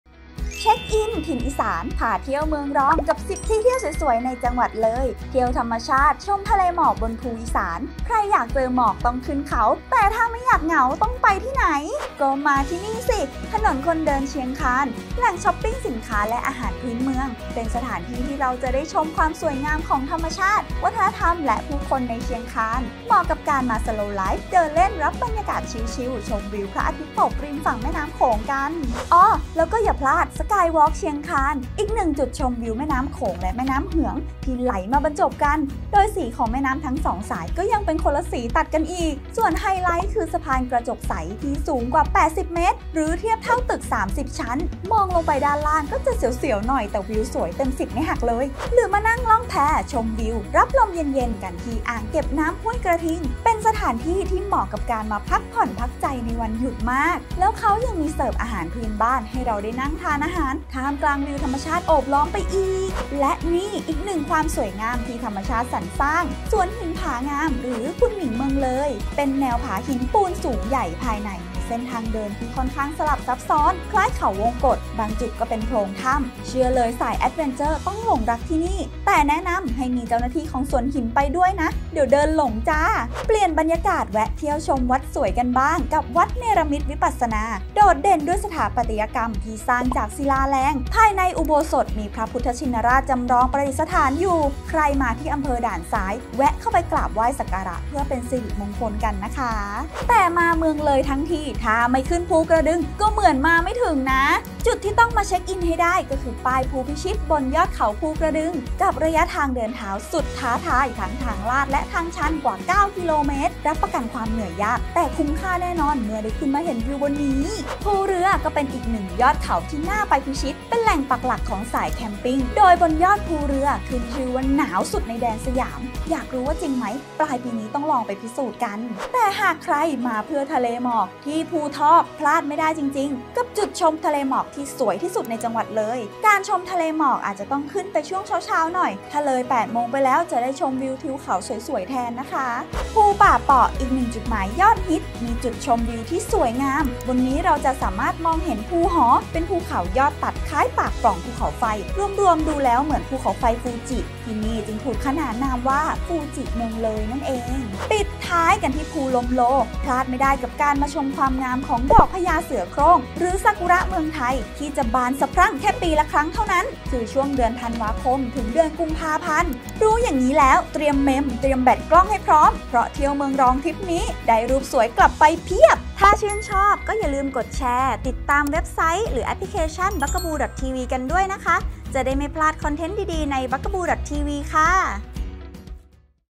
10 ที่เที่ยวเลย เชียงคาน เที่ยวเมืองรอง ชมธรรมชาติ ทะเลหมอกภูอีสาน | Bugaboo Travel | Ep.23
เที่ยวเมืองรองลองรักษ์ดู พา เที่ยวจังหวัดเลย เป็นอีกหนึ่งจังหวัดเมืองรองที่มีสถานที่เที่ยวน่าสนใจ ไม่ว่าจะเป็น เชียงคาน ภูกระดึง ภูเรือ ภูลมโล วันนี้เราขอพาเพื่อน ๆ ไปเช็กอินถิ่นอีสานเมืองรอง กับ 10 ที่เที่ยวเลย เชียงคาน เที่ยวเมืองรอง ชมธรรมชาติ ทะเลหมอกภูอีสาน จะมีที่ไหนบ้าง ตามมาดูเลย

1. ถนนคนเดินเชียงคาน • อ.เชียงคาน
2. สกายวอล์ค เชียงคาน • อ.เชียงคาน
3. อ่างเก็บน้ำห้วยกระทิง • อ.เมือง
4. สวนหินผางาม • อ.หนองหิน
5. วัดเนรมิตวิปัสสนา • อ.ด่านซ้าย
6. อุทยานแห่งชาติภูกระดึง • อ.ภูกระดึง

กิจกรรม จับผิดภาพ Photo Hunt